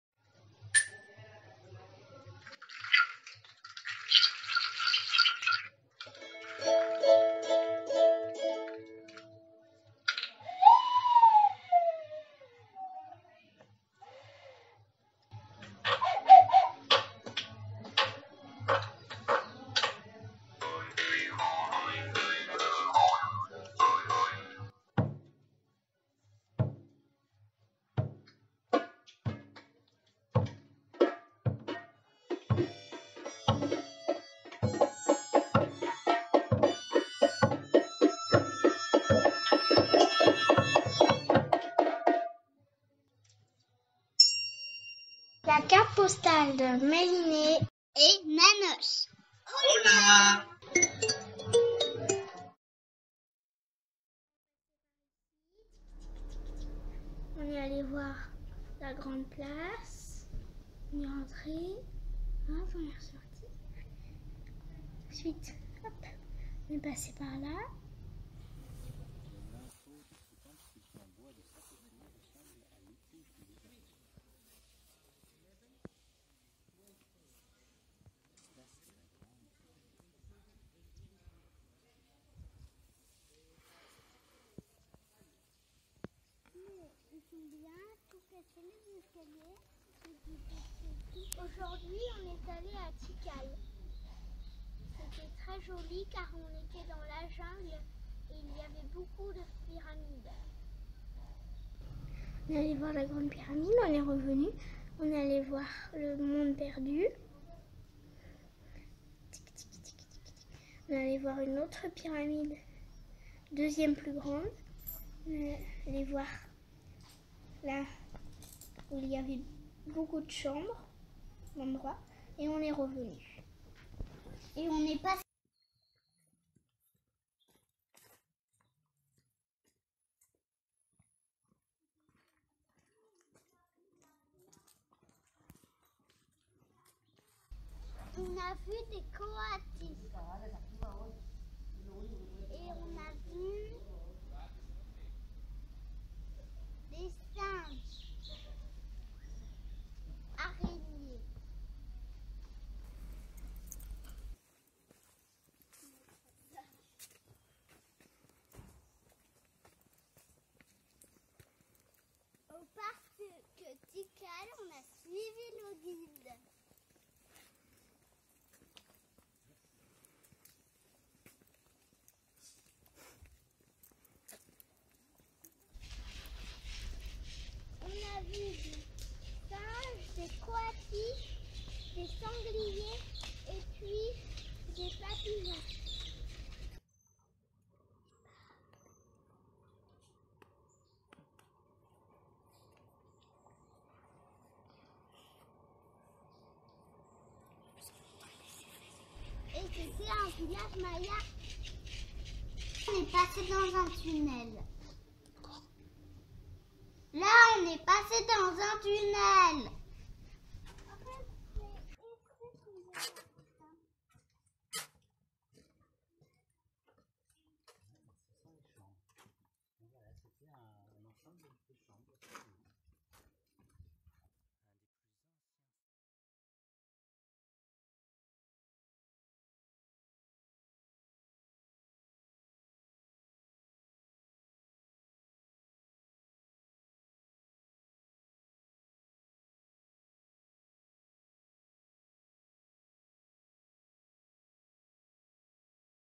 Carte postale de Mélinée et Nanosh #4
Présentation non exhaustive de Tikal dans la région de Petén au Guatemala, réalisée lors de notre grand voyage en Amérique en 2019.